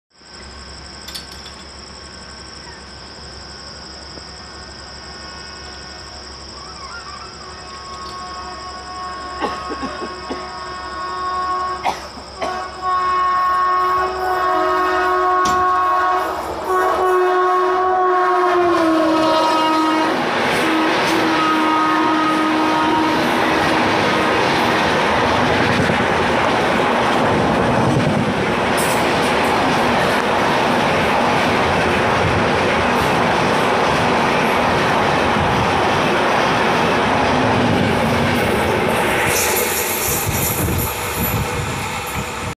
चौरी चौरा ने बिजली का ⚡ हाई वोल्ट का झटका दे कर साइड किया मऊ राजधानी को
चौरी चौरा ने बिजली का हाई वोल्ट का झटका दे कर साइड किया मऊ राजधानी को.. 15003 V/S 22539